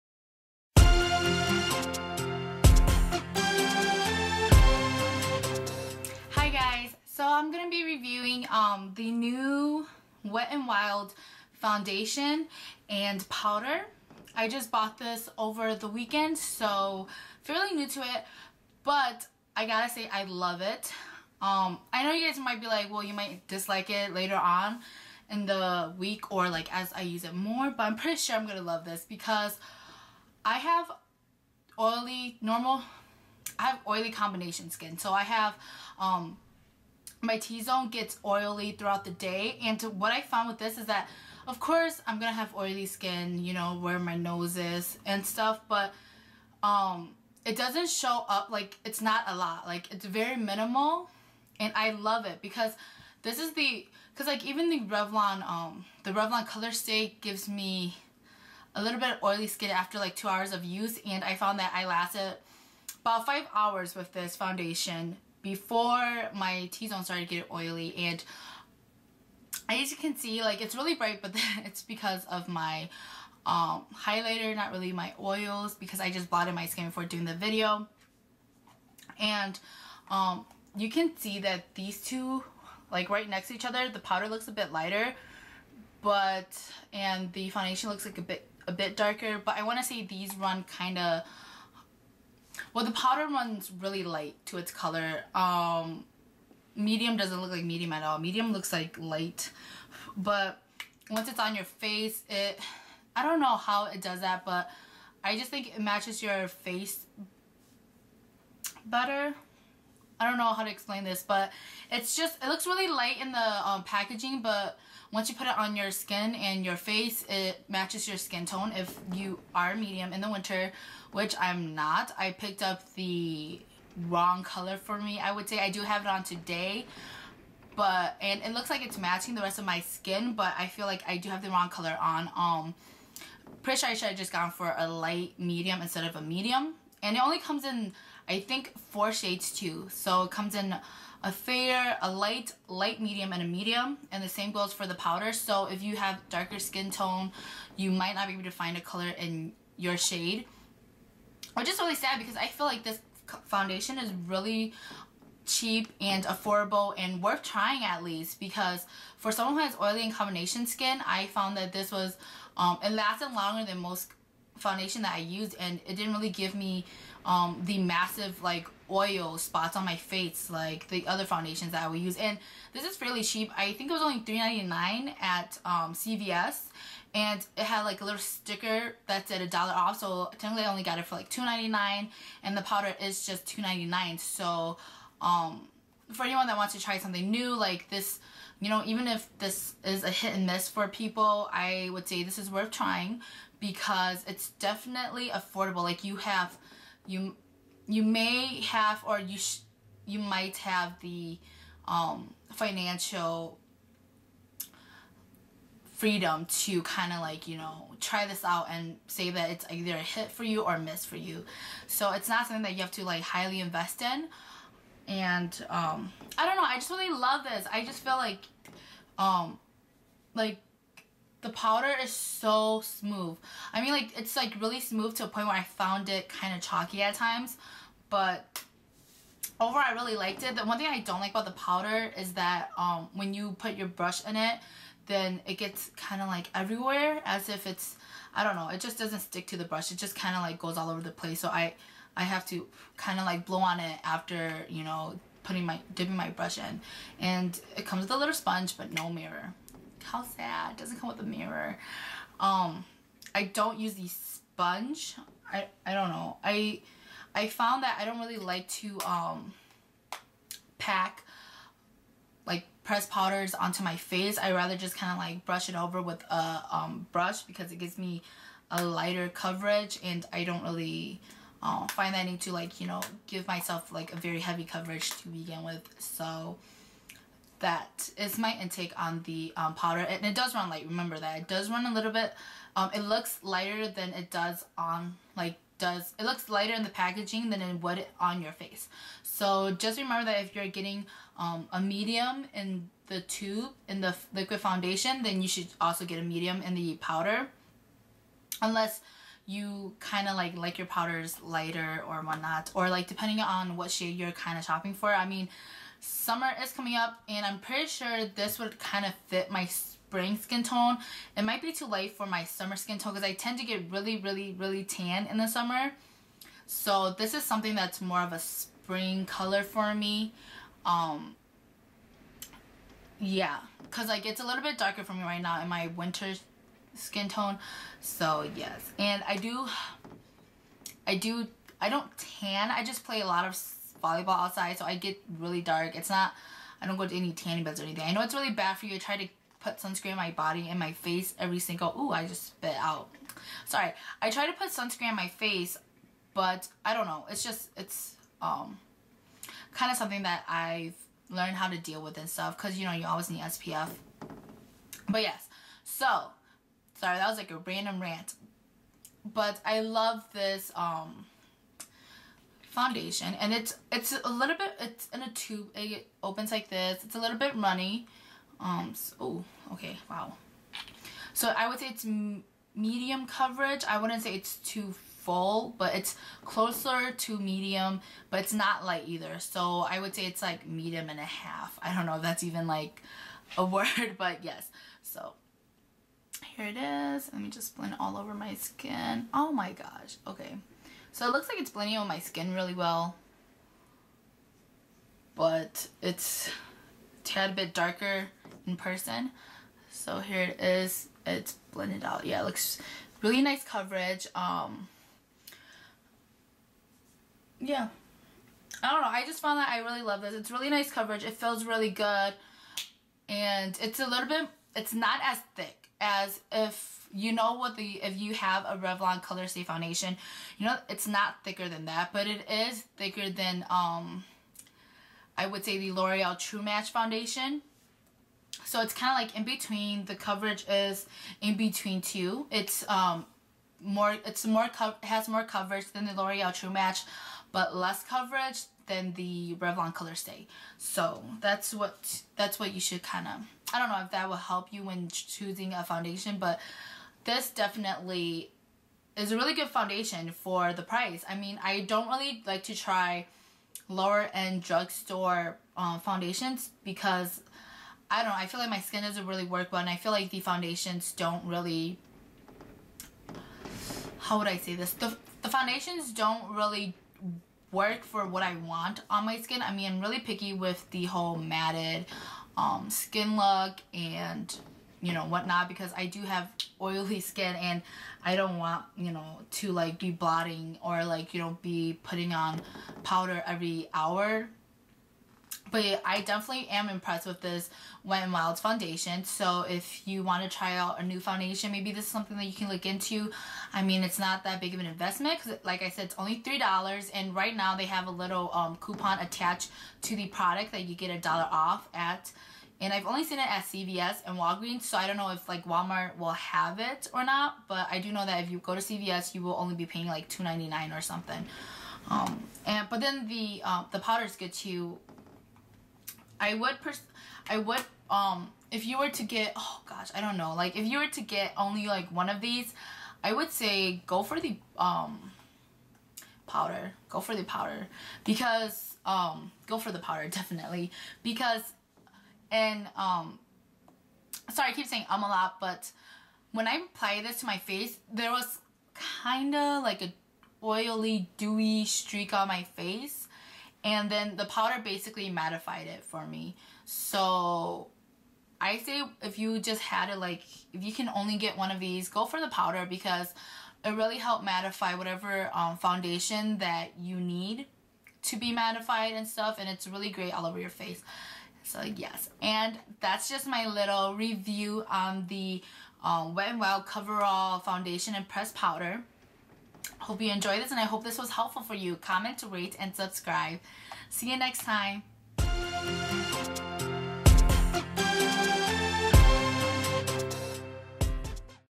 Review: Wet n Wild Coverall Foundation and Powder
Hello Youtube!
Here's my review on the new Wet n Wild Foundation and Powder!

What I have on:
Cardigan - Forever 21

Face Makeup:
Face Primer - Salley Hansen Organic Face Primer
Foundation - Wet n Wild Coverall in Medium
Powder - Wet n Wild Coverall in Medium
Bronzer - Milani Compressed Mineral Powder in Tan & Rimmel Bronzer in Sun Tan
Highlight - Wet n Wild Bronzer in Reserve Your Cabana
Concealer - Maybelline Age Rewind Concealer in Honey & Revlon Photoready Concealer in Medium
Eyes - NYX Cavier and Bubbles Neutral Eye Palette
Eye Liner - Maybelline Eye Studio Gel Liner in Blackest Black & Essence Pencil Liner in  Black
Lips - No.7 in Ginger Mousse
Eyebrows - Beauty Rush Eyeshadow Duo
Lashes - Covergirl NatureLux Mascara in Blackest Black & Almay Get up and Grow in Black